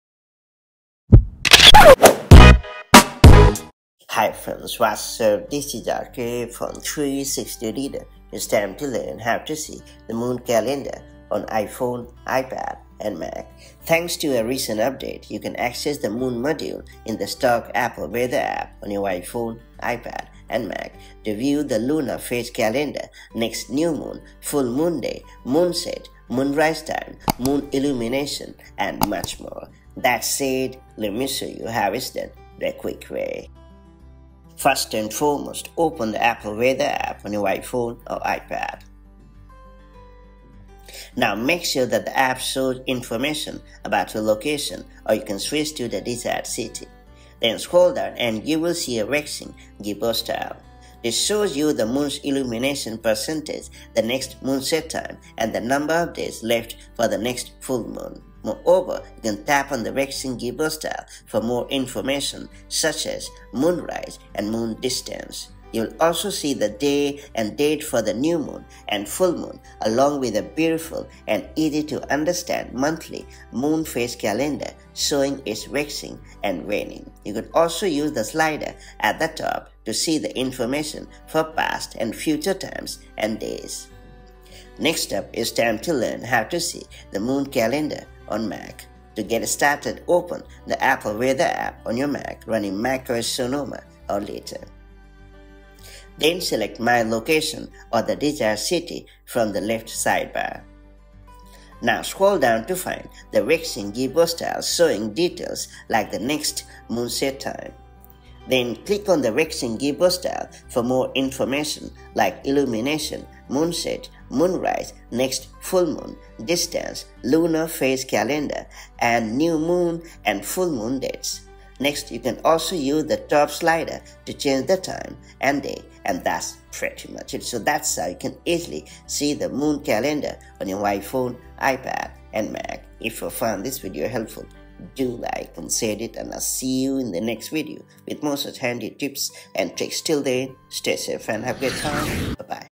How to See Moon Calendar on iPhone, iPad, and Mac
Thanks to the introduction of iOS 17, iPadOS 17, and macOS 14 Sonoma, it has become possible to view the moon calendar on iPhone, iPad, and Mac. Watch this video to learn how it's done: